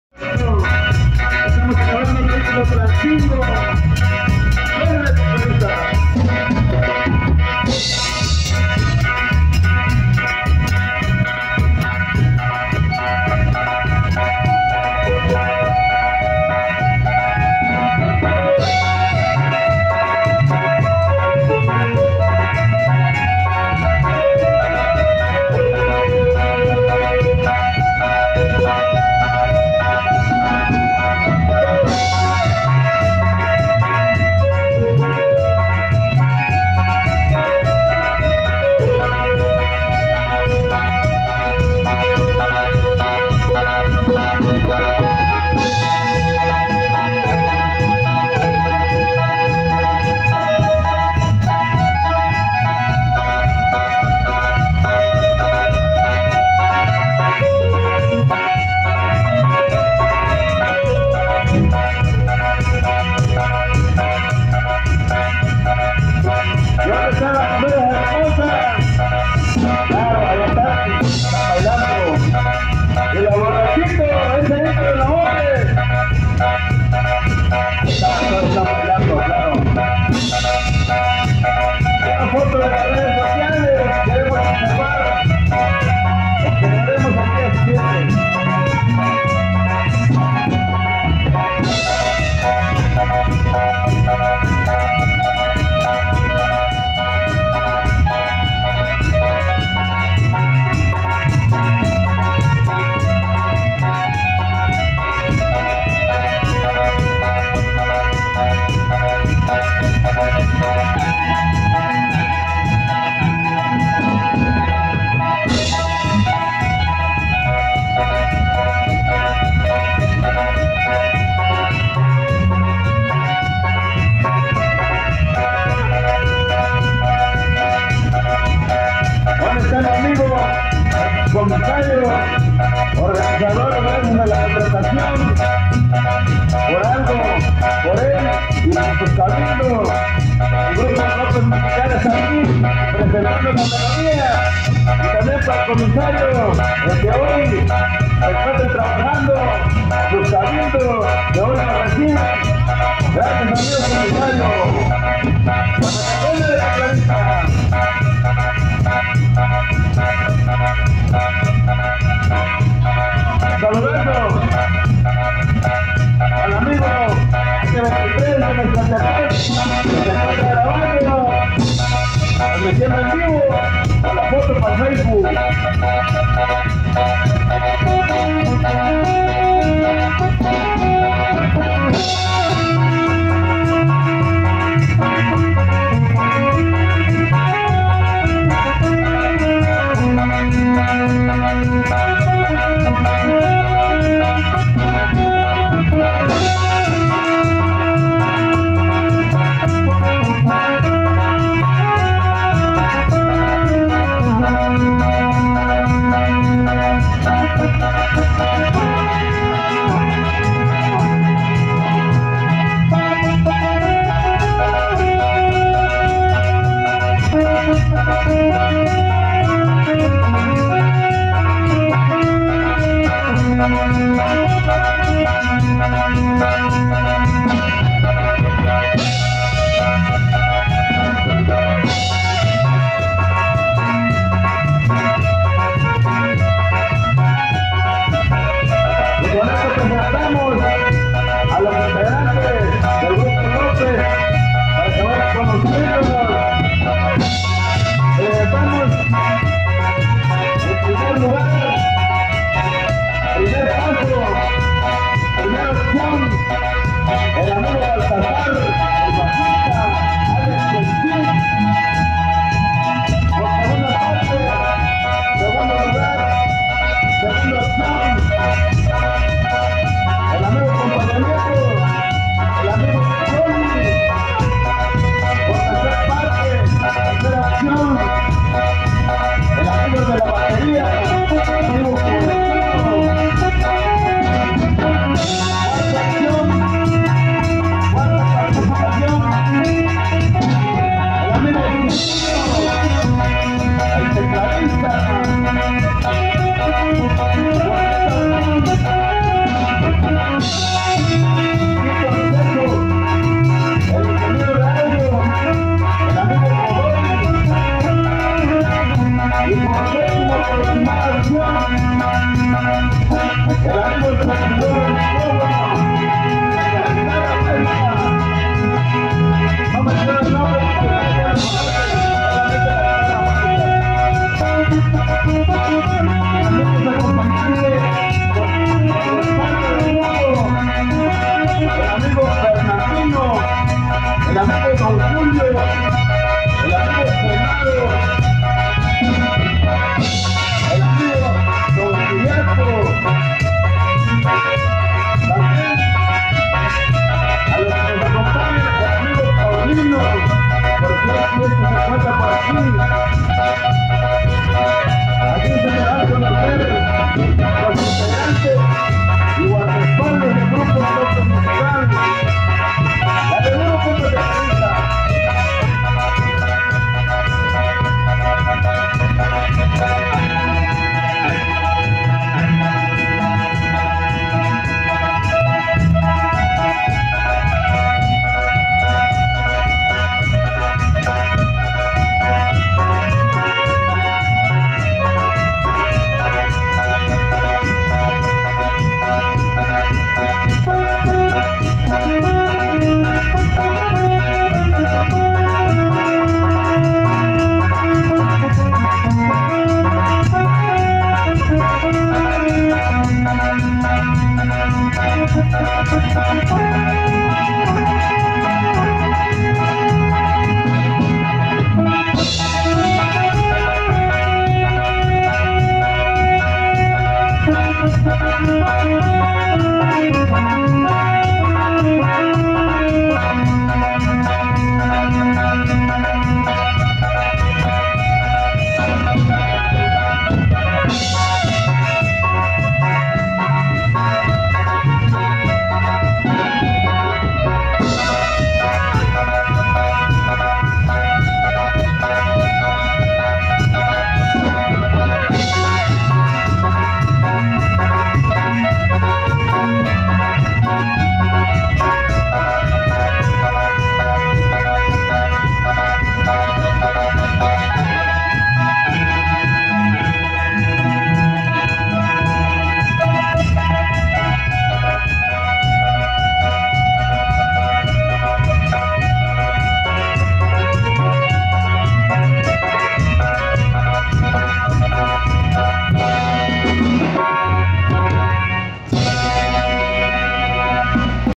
López Musical en xilostlancingo (chilenas)
baile en xilostlancingo municipio de acatepec guerrero el 29 de enero 2022